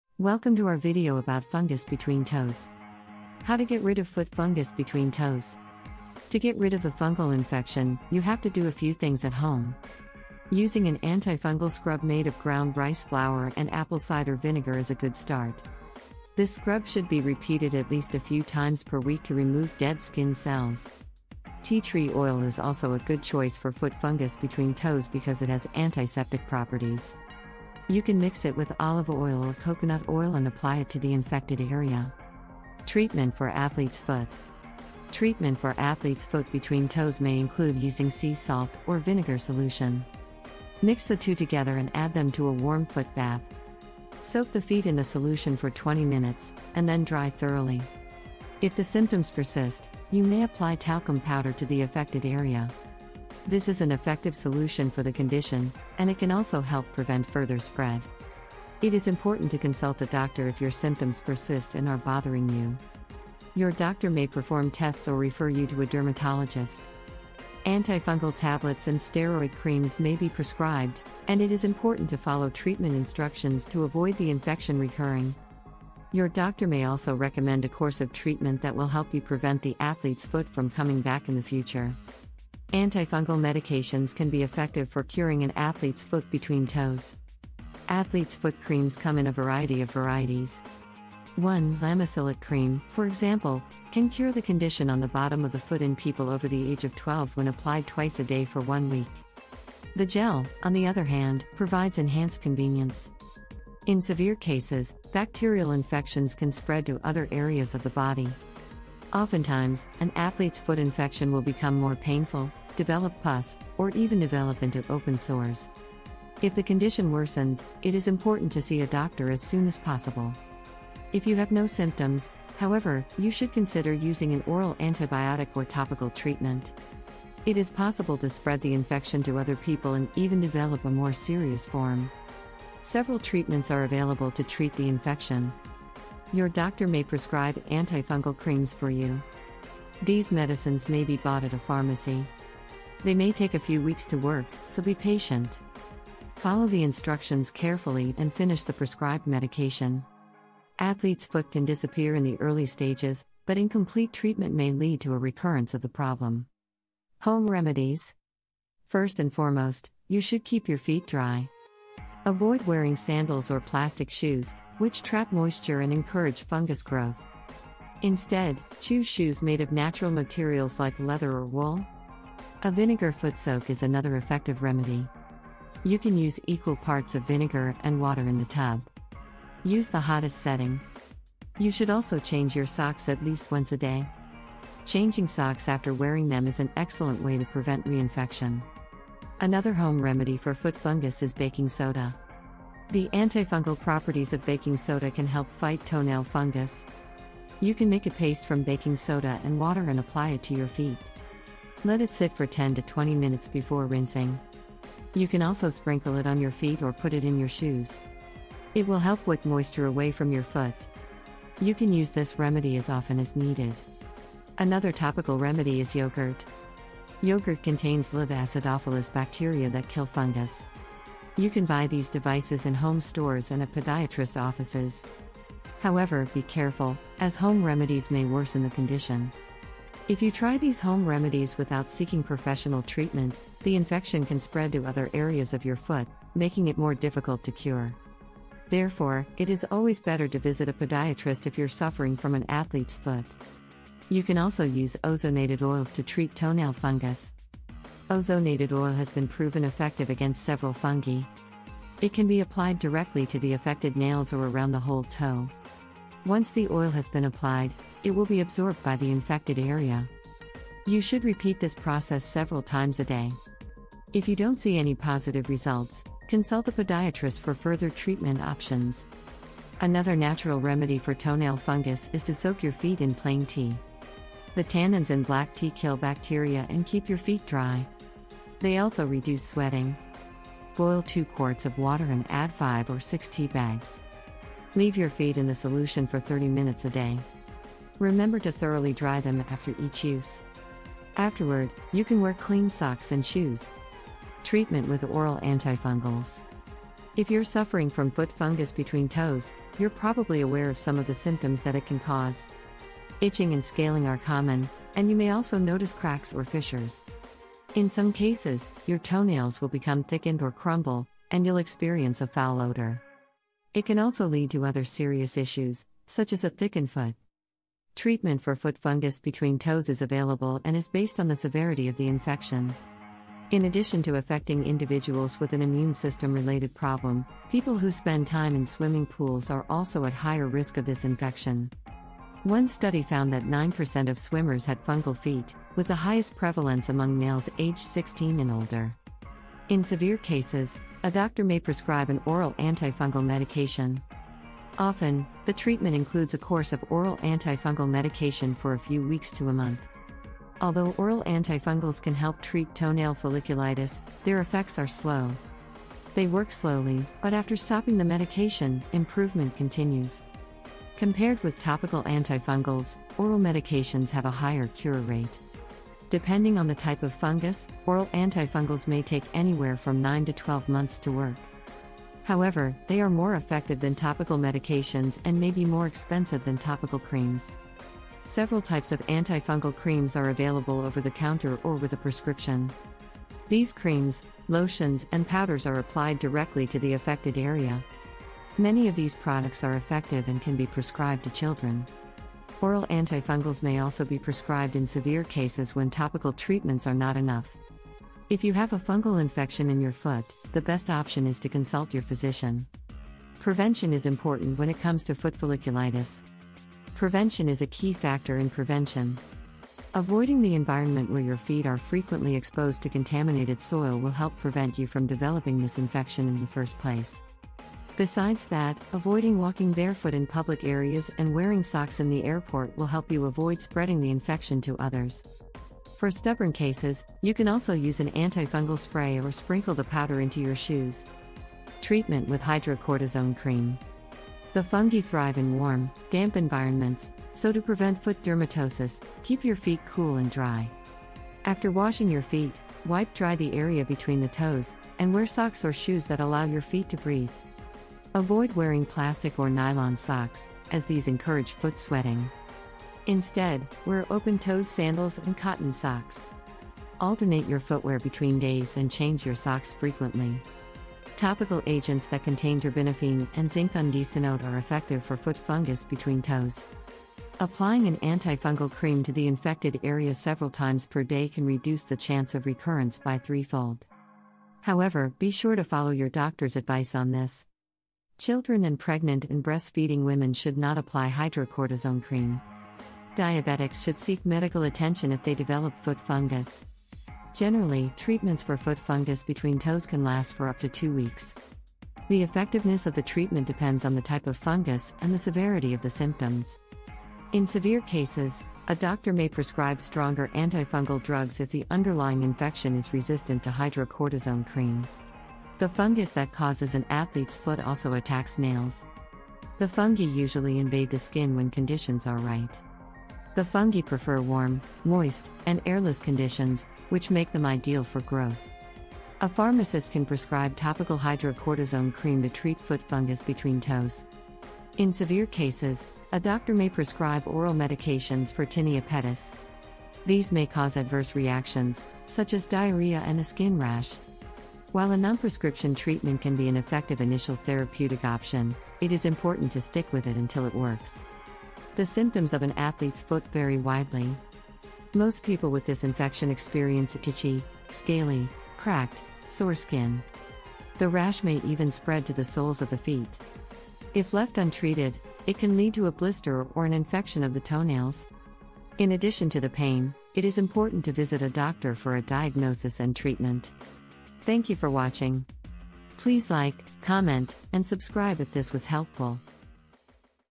How to Get Rid of Foot Fungus Between Toes
To get rid of the fungal infection, you have to do a few things at home. Using an antifungal scrub made of ground rice flour and apple cider vinegar is a good start. This scrub should be repeated at least a few times per week to remove dead skin cells. Tea tree oil is also a good choice for foot fungus between toes because it has antiseptic properties. You can mix it with olive oil or coconut oil and apply it to the infected area.

Treatment for athlete's foot
Treatment for athlete's foot between toes may include using sea salt or vinegar solution. Mix the two together and add them to a warm foot bath. Soak the feet in the solution for 20 minutes, and then dry thoroughly. If the symptoms persist, you may apply talcum powder to the affected area. This is an effective solution for the condition, and it can also help prevent further spread.

It is important to consult a doctor if your symptoms persist and are bothering you. Your doctor may perform tests or refer you to a dermatologist. Antifungal tablets and steroid creams may be prescribed, and it is important to follow treatment instructions to avoid the infection recurring. Your doctor may also recommend a course of treatment that will help you prevent the athlete's foot from coming back in the future.

Antifungal medications can be effective for curing an athlete's foot between toes. Athlete's foot creams come in a variety of varieties. One, Lamisil AT Cream, for example, can cure the condition on the bottom of the foot in people over the age of 12 when applied twice a day for one week. The gel, on the other hand, provides enhanced convenience.

In severe cases, bacterial infections can spread to other areas of the body. Oftentimes, an athlete's foot infection will become more painful, develop pus, or even develop into open sores. If the condition worsens, it is important to see a doctor as soon as possible. If you have no symptoms, however, you should consider using an oral antibiotic or topical treatment. It is possible to spread the infection to other people and even develop a more serious form.

Several treatments are available to treat the infection. Your doctor may prescribe antifungal creams for you. These medicines may be bought at a pharmacy. They may take a few weeks to work, so be patient. Follow the instructions carefully and finish the prescribed medication. Athlete's foot can disappear in the early stages, but incomplete treatment may lead to a recurrence of the problem.

Home remedies
First and foremost, you should keep your feet dry. Avoid wearing sandals or plastic shoes, which trap moisture and encourage fungus growth. Instead, choose shoes made of natural materials like leather or wool. A vinegar foot soak is another effective remedy. You can use equal parts of vinegar and water in the tub. Use the hottest setting. You should also change your socks at least once a day. Changing socks after wearing them is an excellent way to prevent reinfection.

Another home remedy for foot fungus is baking soda. The antifungal properties of baking soda can help fight toenail fungus. You can make a paste from baking soda and water and apply it to your feet. Let it sit for ten to twenty minutes before rinsing. You can also sprinkle it on your feet or put it in your shoes. It will help wick moisture away from your foot. You can use this remedy as often as needed.

Another topical remedy is yogurt. Yogurt contains live acidophilus bacteria that kill fungus. You can buy these devices in home stores and at podiatrist offices. However, be careful, as home remedies may worsen the condition. If you try these home remedies without seeking professional treatment, the infection can spread to other areas of your foot, making it more difficult to cure. Therefore, it is always better to visit a podiatrist if you're suffering from an athlete's foot.

You can also use ozonated oils to treat toenail fungus. Ozonated oil has been proven effective against several fungi. It can be applied directly to the affected nails or around the whole toe. Once the oil has been applied, it will be absorbed by the infected area. You should repeat this process several times a day. If you don't see any positive results, consult a podiatrist for further treatment options.

Another natural remedy for toenail fungus is to soak your feet in plain tea. The tannins in black tea kill bacteria and keep your feet dry. They also reduce sweating. Boil two quarts of water and add five or six tea bags. Leave your feet in the solution for 30 minutes a day. Remember to thoroughly dry them after each use. Afterward, you can wear clean socks and shoes.

Treatment with oral antifungals
If you're suffering from foot fungus between toes, you're probably aware of some of the symptoms that it can cause.